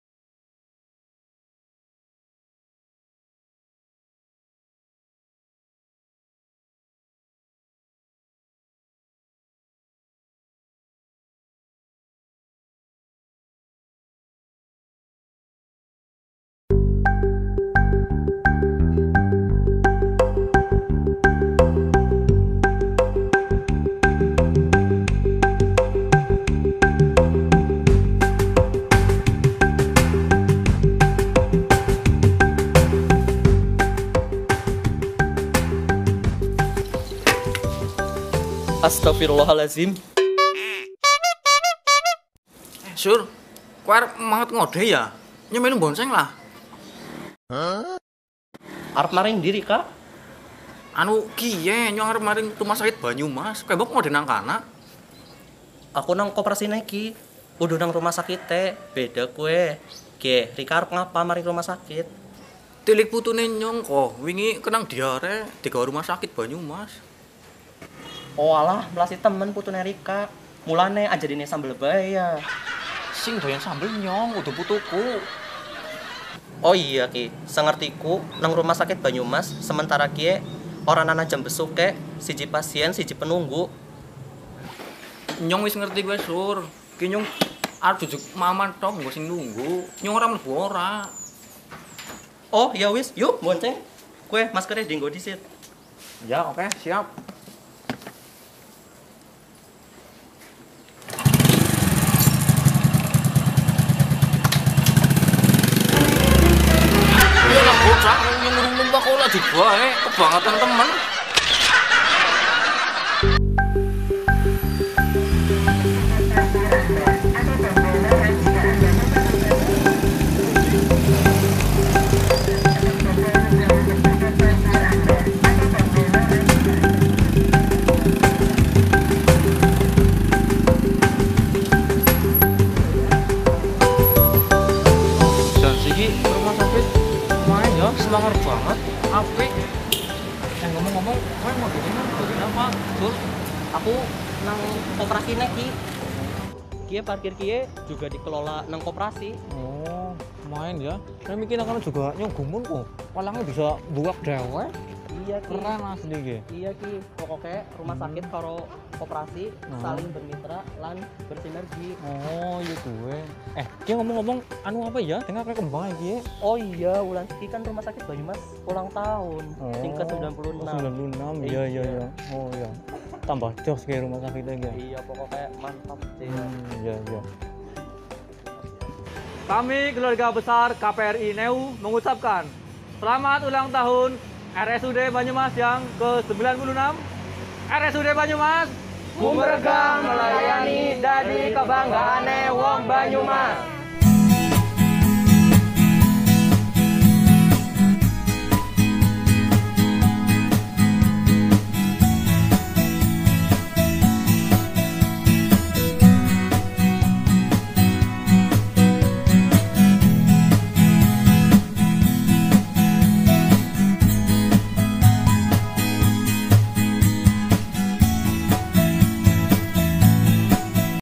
Ucapan Ulang Tahun RSUD Banyumas dari Koperasi NEU
Video ucapan dari Koperasi NUE RSUD Banyumas

Dirgahayu RSUD Banyumas yang ke 96
Gumregah Melayani Dadi Kebanggane Wong Banyumas

Terima kasih atas ucapannya
Manajemen RSUD Banyumas